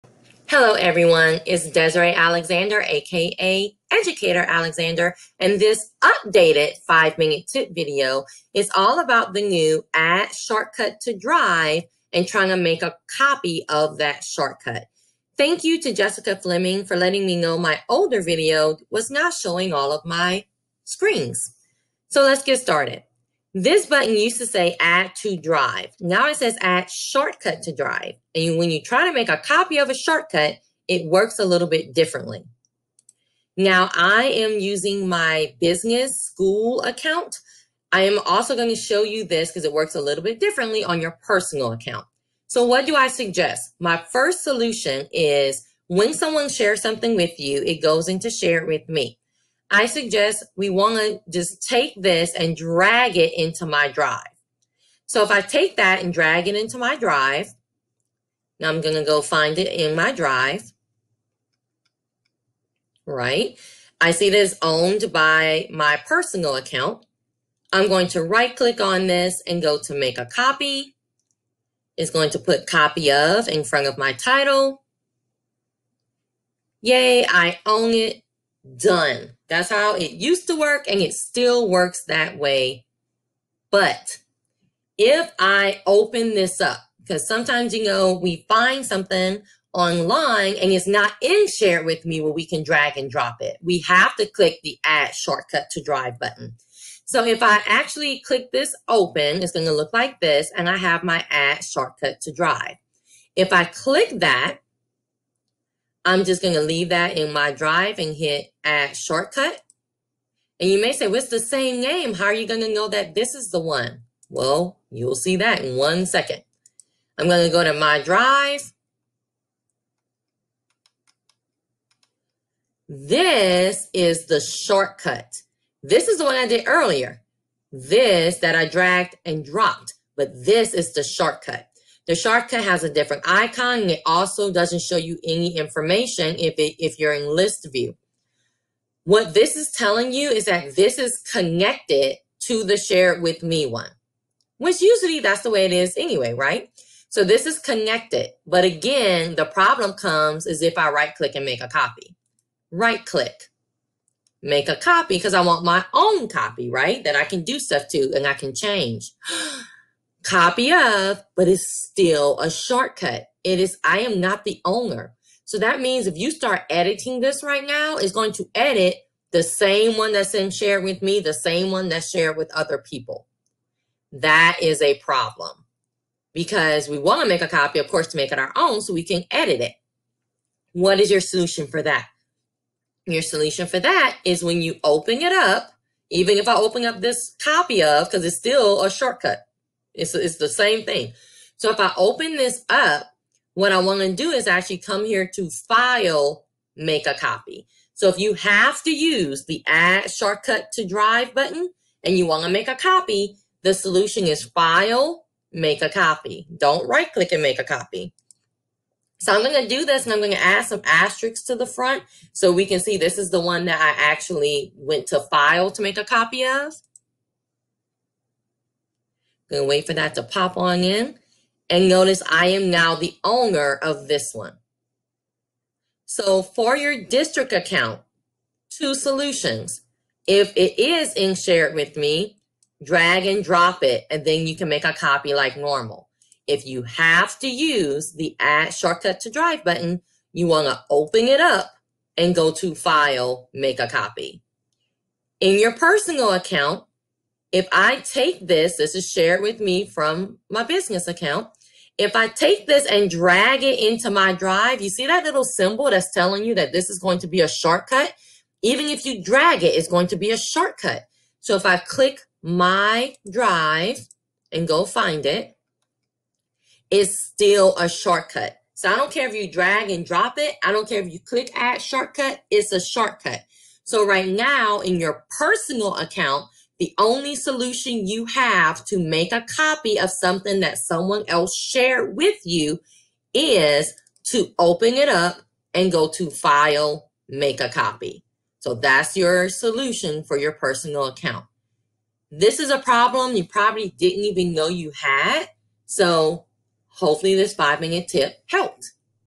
Updated: New Google Add Shortcut to Drive Fix to Copy Problem
Learn how to navigate the new ADD SHORTCUT TO DRIVE in Google Drive and how to fix the copy problem you may not have noticed yet!

to sign up for the monthly newsletter, follow on Twitter, Like on Facebook or follow in Instagram.